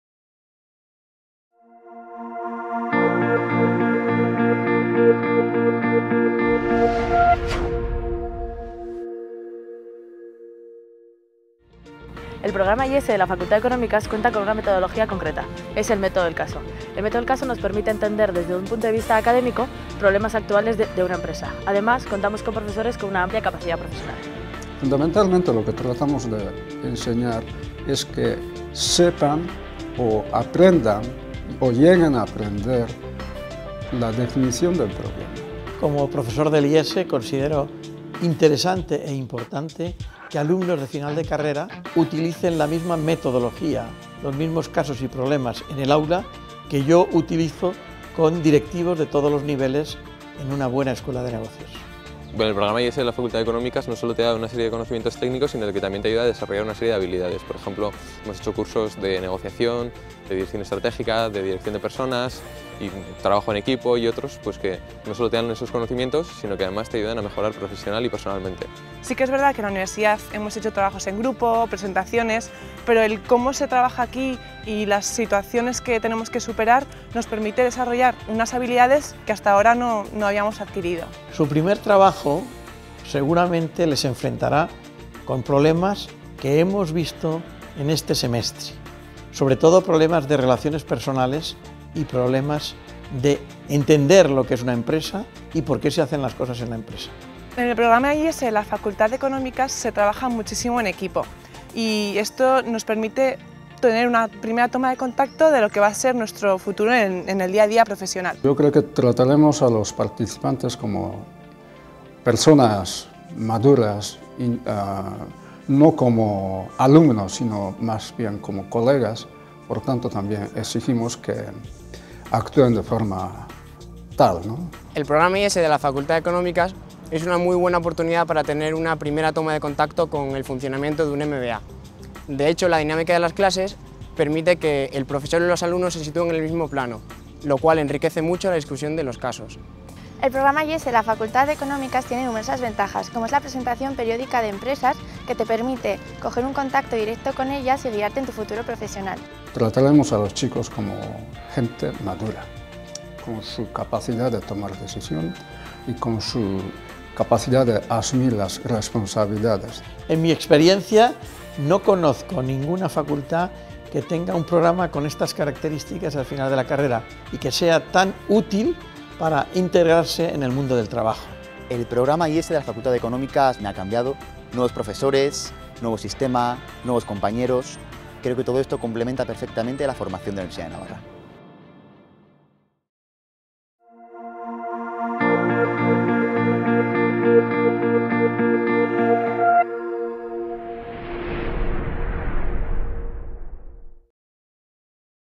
Programa IESE. Facultad de Económicas de la Universidad de Navarra
El Programa IESE de la Facultad de Económicas de la Universidad de Navarra es el único programa de grado integrado en una Business School de primer nivel.